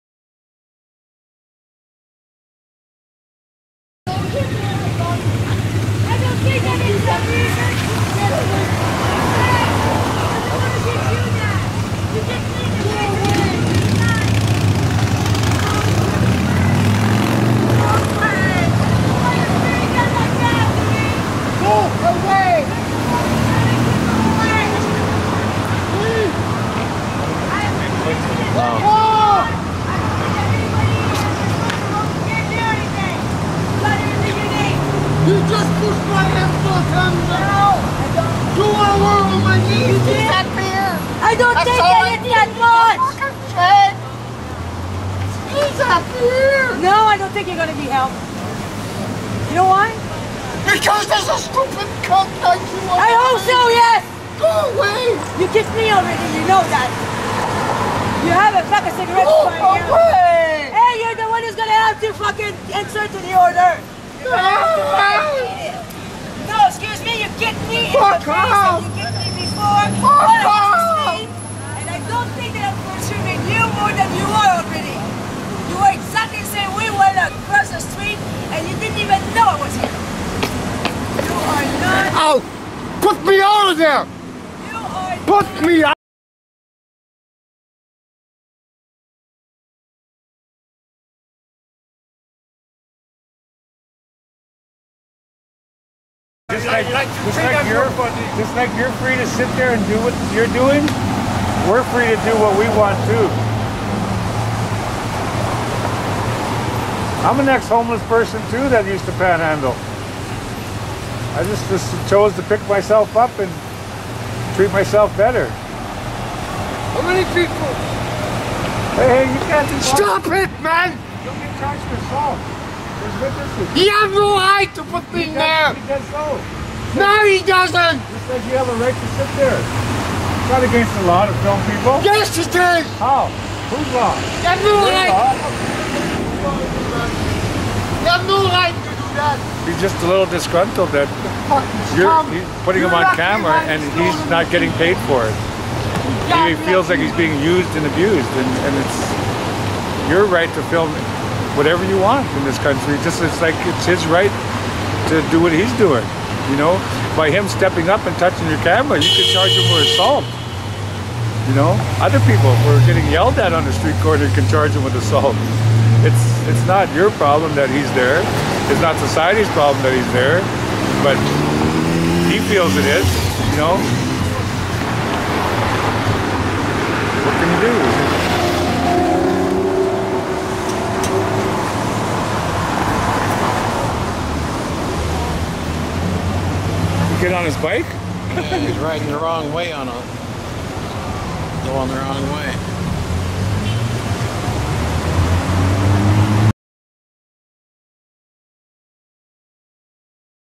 first pan handler - assaults camera
Vancouver pan Handlers thinks he has rights of privacy when in public. The native man is my guide that narrates the scene.